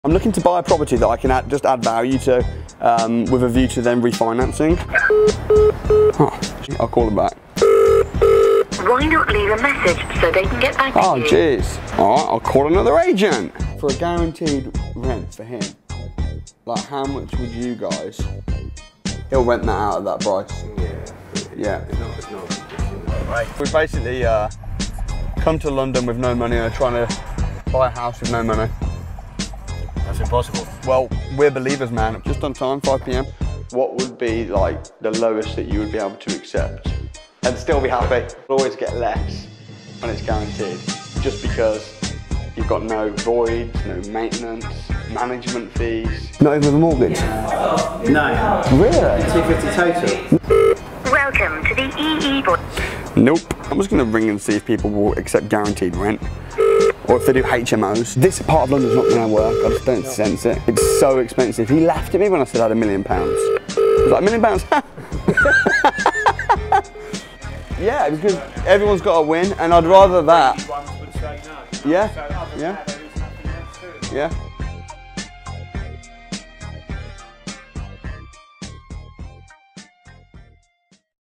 My next video is ...
It’s coming on 26.12.19 at 7pm. I attempt my hardest challenge to date - securing a no money down property deal in London within 24 hours.